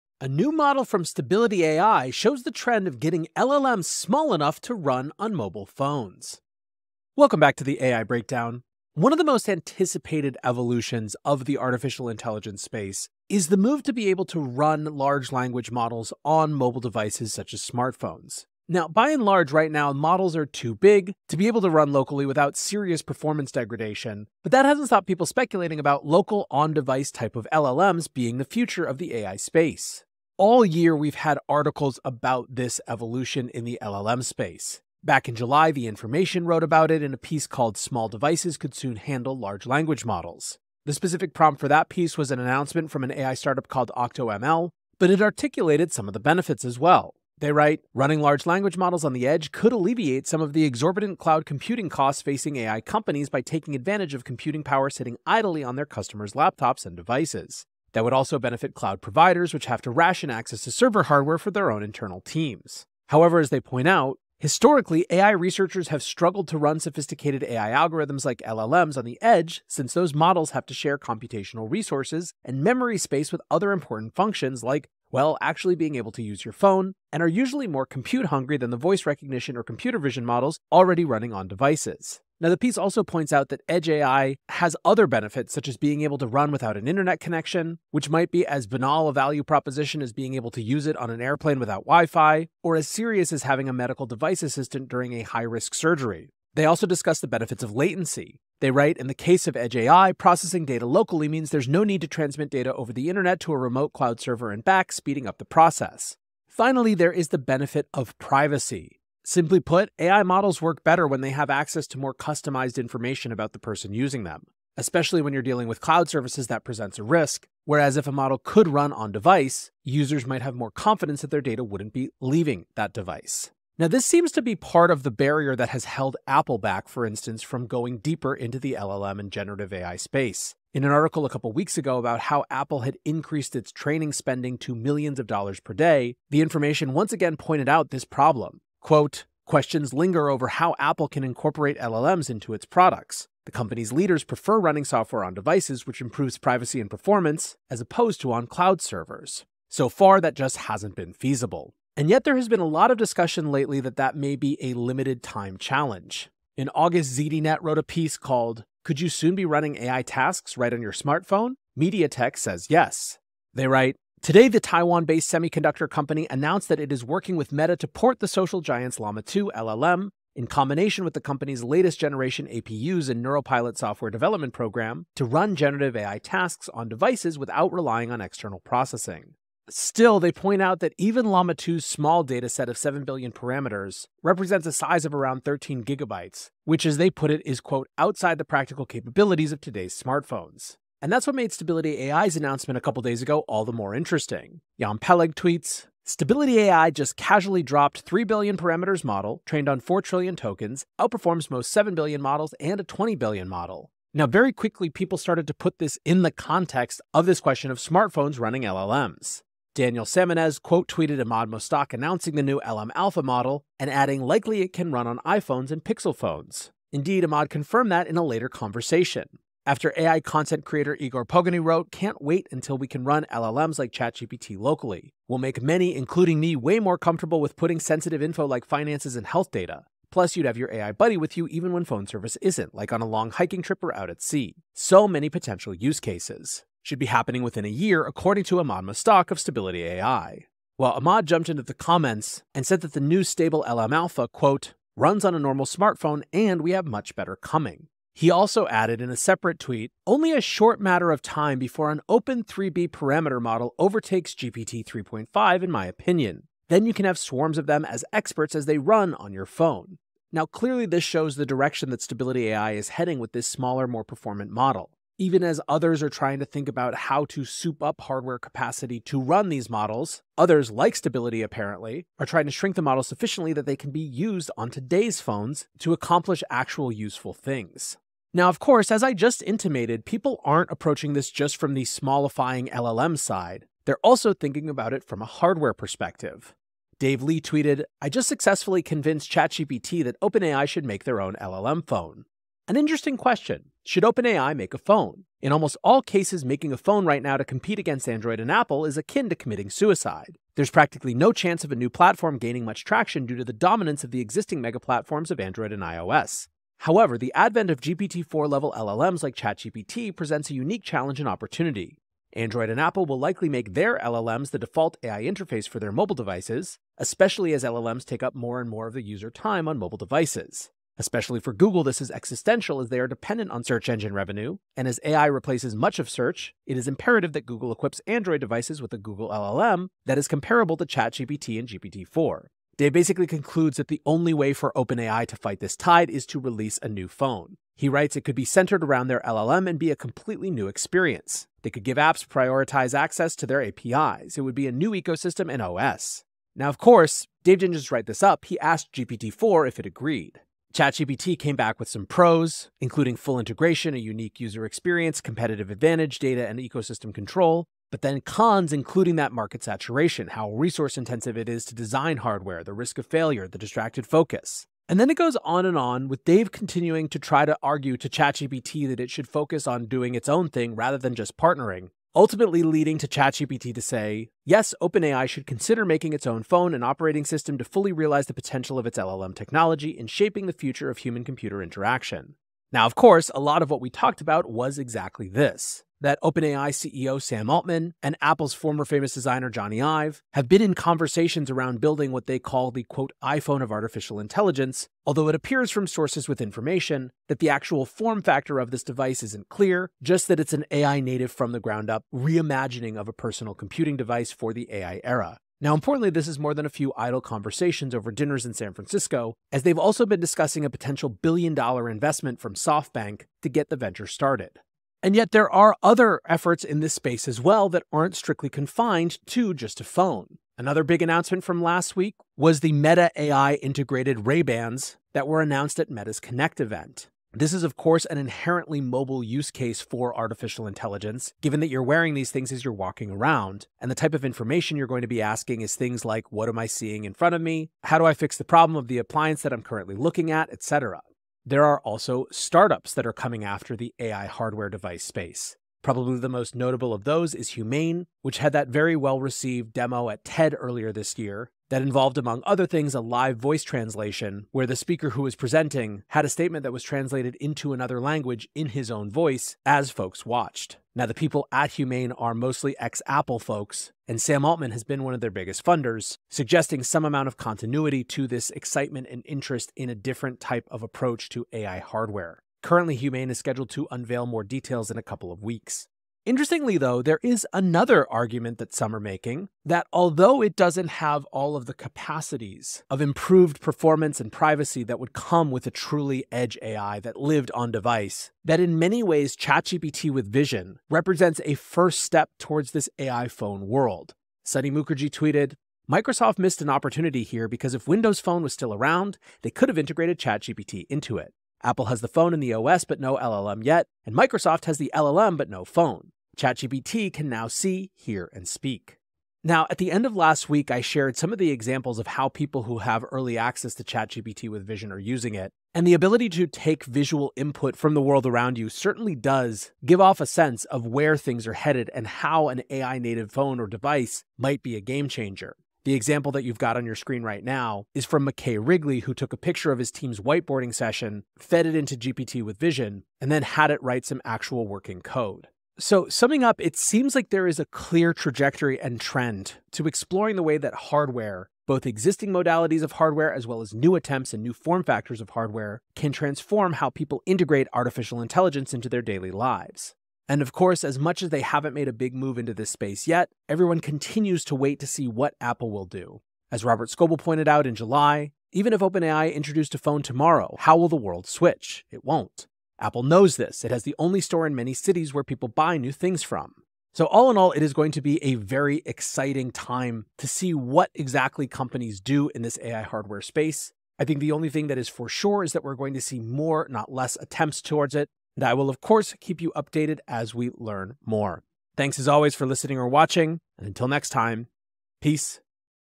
Mobile LLMs? The Battle to Get Edge AI on Our Phones
Stability AI has released a new 3b model that outperforms some 7b and 20b models, and previews the forthcoming battle to get local LLMs on our mobile devices.

0:00 Context
0:37 LLMs on small devices
2:33 A limited time challenge?
3:14 StabilityAI's announcement
4:38 Should OpenAI make a phone?
6:54 Other players
7:56 ChatGPT Vision is the 1st step
8:57 Summing up